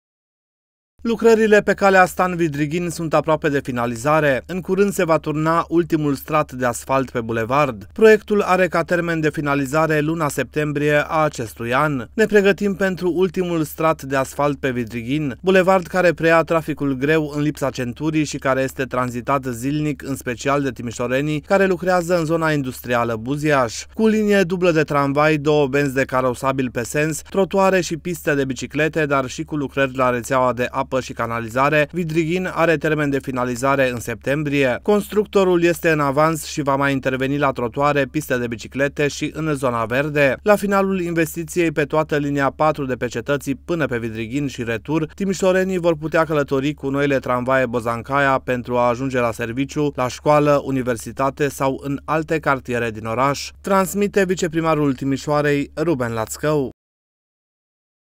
BULEVARDUL STAN VIDRIGHIN, APROAPE DE FINALIZARE
Lucrările pe Calea Stan Vidrighin sunt aproape de finalizare. În curând se va turna ultimul strat de asflat pe bulverd. Proiectul are ca termen de finalizare luna septembrie a acestui an. ”Ne pregătim pentru ultimul strat de asfalt pe Vidrighin, bulevard care preia traficul greu, în lipsa centurii, și care este tranzitat zilnic, în special, de timișorenii care lucrează în Zona Industrială Buziaș.